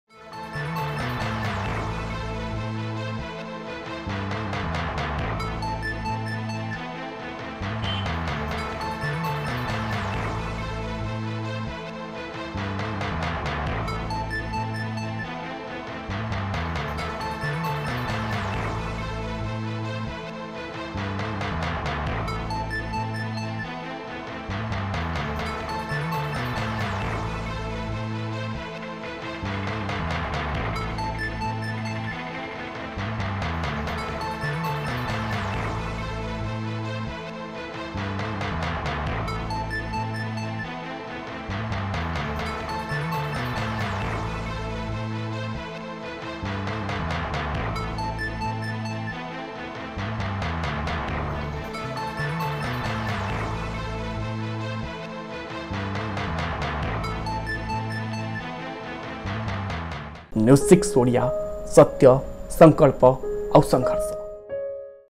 ମେସିନ ଭାଙ୍ଗି ଲୁଟି ନେଇଛନ୍ତି ସମସ୍ତ ଟଙ୍କା ।News6odia
ନାଚୁଣୀ ବଜାରର ଏଚଡ଼ିଏଫସି ବ୍ୟାଙ୍କ ଏଟିଏମରୁ ଦୁର୍ବୃତ୍ତମାନେ ମେସିନ ଭାଙ୍ଗି  ଲୁଟି ନେଇଛନ୍ତି ସମସ୍ତ ଟଙ୍କା ।